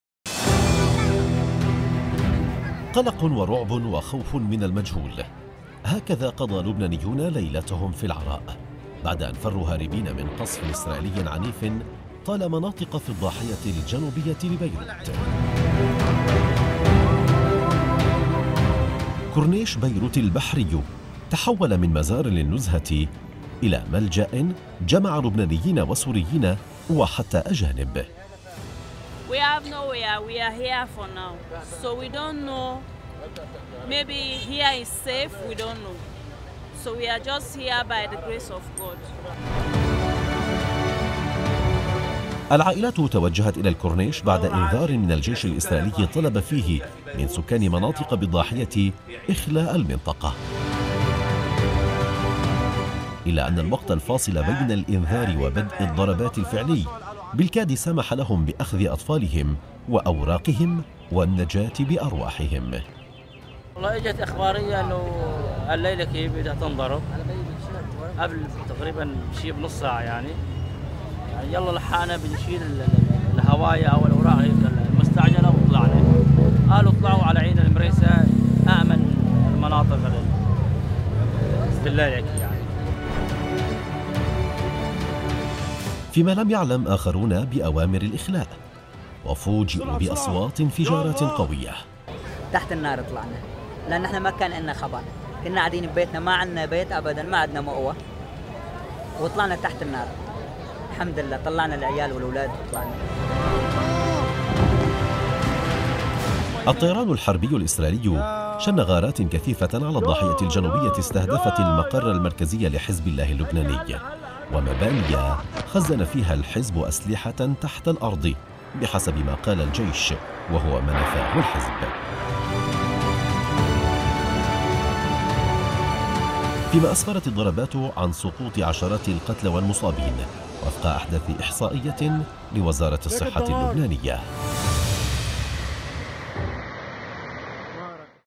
هرباً من الغارات الإسرائيلية.. لبنانيون وسوريون وأجانب يقضون ليلتهم في كورنيش بيروت
قلق ورعب وخوف من المجهول.. لبنانيون وسوريون وأجانب يقضون ليلتهم في كورنيش بيروت بعد ليلة عنيفة من القصف الإسرائيلي

لمعرفة أحدث المستجدات.. عبر البث المباشر من هنا
وبإمكانكم متابعة الحدث على منصات التواصل